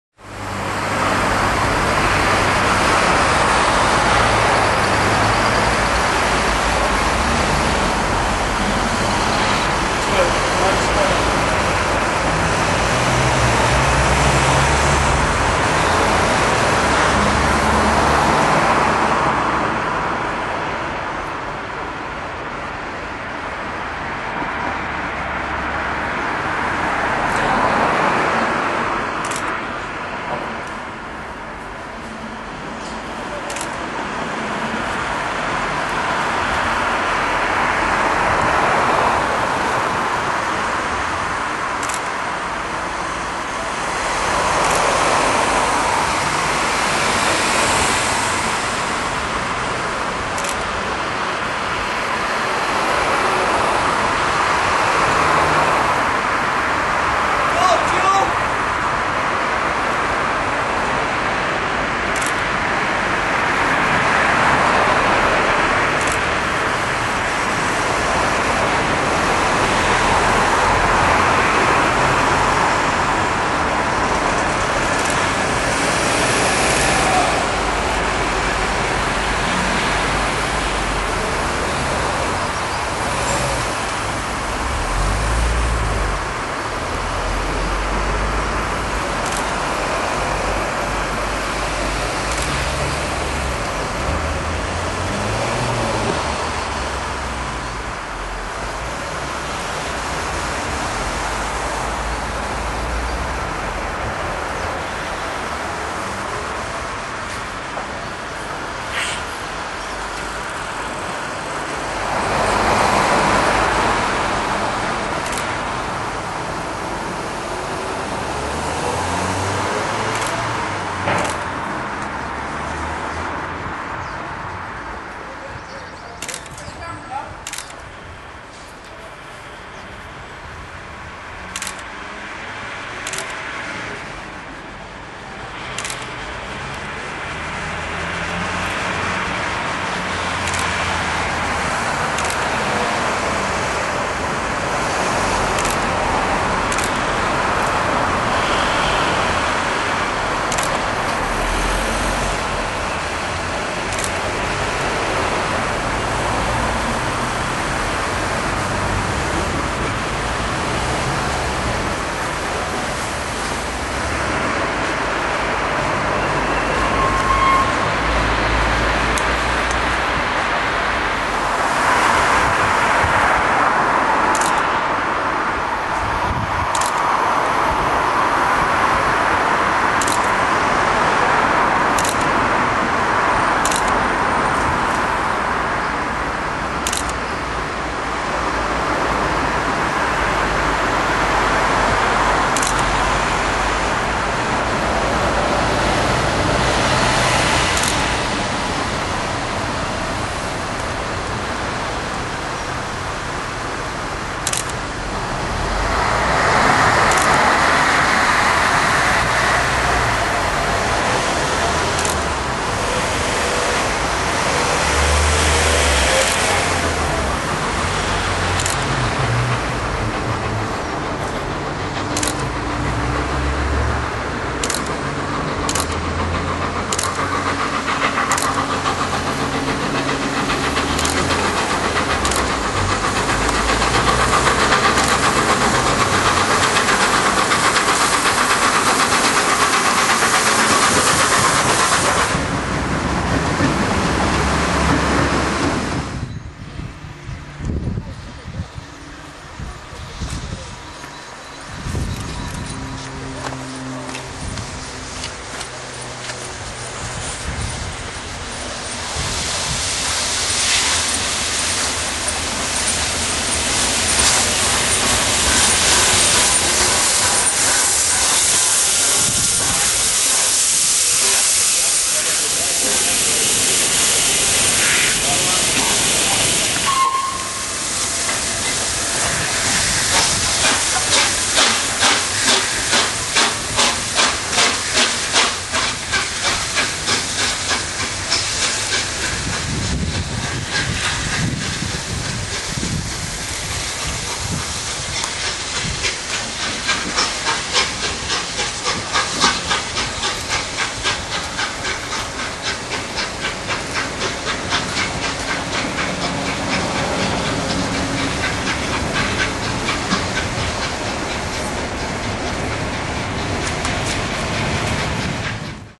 Torando No 60163 - Royal Train Thursday 19th February 2009
Torando No 60163 - THe official naming of the loco with the Royal Train Thursday 19th February 2009 at York and Church Fenton